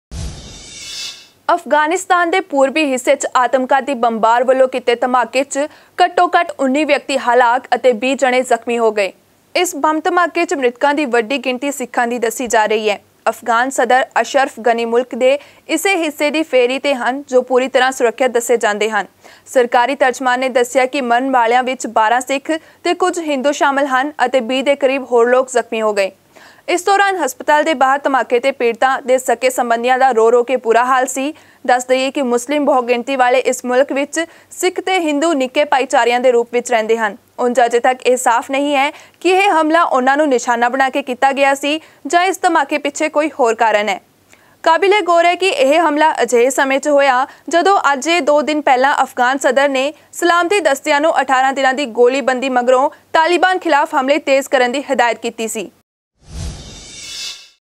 SUICIDE BOMBER KILLS SIKHS IN AFGHANISTAN ਅਫ਼ਗ਼ਾਨਿਸਤਾਨ | SIKH TV
ਅਫ਼ਗ਼ਾਨਿਸਤਾਨ ਦੇ ਪੂਰਬੀ ਹਿੱਸੇ 'ਚ ਆਤਮਘਾਤੀ ਬੰਬਾਰ ਵੱਲੋਂ ਕੀਤੇ ਧਮਾਕੇ 'ਚ ਘੱਟੋ ਘੱਟ 19 ਵਿਅਕਤੀ ਹਲਾਕ ਅਤੇ 20 ਜਣੇ ਜ਼ਖ਼ਮੀ ਹੋ ਗਏ।
ਇਸ ਬੰਬ ਧਮਾਕੇ ਚ ਮ੍ਰਿਤਕਾਂ ਦੀ ਵੱਡੀ ਗਿਣਤੀ ਸਿੱਖਾਂ ਦੀ ਦੱਸੀ ਜਾ ਰਹੀ ਹੈ । ਅਫ਼ਗ਼ਾਨ ਸਦਰ ਅਸ਼ਰਫ਼ ਗ਼ਨੀ ਮੁਲਕ ਦੇ ਇਸੇ ਹਿੱਸੇ ਦੀ ਫ਼ੇਰੀ 'ਤੇ ਹਨ, ਜੋ ਪੂਰੀ ਤਰ੍ਹਾਂ ਸੁਰੱਖਿਅਤ ਦੱਸੇ ਜਾਂਦੇ ਹਨ। ਸਰਕਾਰੀ ਤਰਜਮਾਨ ਨੇ ਦੱਸਿਆ ਕਿ ਮਰਨ ਵਾਲਿਆਂ 'ਚ 12 ਸਿੱਖ ਤੇ ਕੁਝ ਹਿੰਦੂ ਸ਼ਾਮਲ ਹਨ ਅਤੇ 20 ਦੇ ਕਰੀਬ ਹੋਰ ਲੋਕ ਜ਼ਖ਼ਮੀ ਹੋ ਗਏ। ਇਸ ਦੌਰਾਨ ਹਸਪਤਾਲ ਦੇ ਬਾਹਰ ਧਮਾਕੇ ਦੇ ਪੀੜਤਾਂ ਦੇ ਸਕੇ-ਸਬੰਧੀਆਂ ਦਾ ਰੋ- ਰੋ ਕੇ ਬੁਰਾ ਹਾਲ ਸੀ। ਦੱਸਦਈਏ ਕਿ ਮੁਸਲਿਮ ਬਹੁਗਿਣਤੀ ਵਾਲੇ ਇਸ ਮੁਲਕ ਵਿੱਚ ਸਿੱਖ ਤੇ ਹਿੰਦੂ ਨਿੱਕੇ ਭਾਈਚਾਰਿਆਂ ਦੇ ਰੂਪ ਵਿੱਚ ਰਹਿੰਦੇ ਹਨ। ਉਂਜ ਅਜੇ ਤਕ ਇਹ ਸਾਫ਼ ਨਹੀਂ ਹੈ ਕਿ ਕੀ ਇਹ ਹਮਲਾ ਉਨ੍ਹਾਂ ਨੂੰ ਨਿਸ਼ਾਨਾ ਬਣਾ ਕੇ ਕੀਤਾ ਗਿਆ ਸੀ ਜਾਂ ਇਸ ਧਮਾਕੇ ਪਿੱਛੇ ਕੋਈ ਹੋਰ ਕਾਰਨ ਹੈ ।ਕਾਬਿਲੇਗੌਰ ਹੈ ਕਿ ਇਹ ਹਮਲਾ ਅਜਿਹੇ ਸਮੇਂ ਹੋਇਆ ਹੈ ਜਦੋਂ ਅਜੇ ਦੋ ਦਿਨ ਪਹਿਲਾਂ ਅਫ਼ਗ਼ਾਨ ਸਦਰ ਨੇ ਸਲਾਮਤੀ ਦਸਤਿਆਂ ਨੂੰ 18 ਦਿਨਾਂ ਦੀ ਗੋਲੀਬੰਦੀ ਮਗਰੋਂ ਤਾਲਿਬਾਨ ਖ਼ਿਲਾਫ਼ ਹਮਲੇ ਤੇਜ਼ ਕਰਨ ਦੀ ਹਦਾਇਤ ਕੀਤੀ ਹੈ।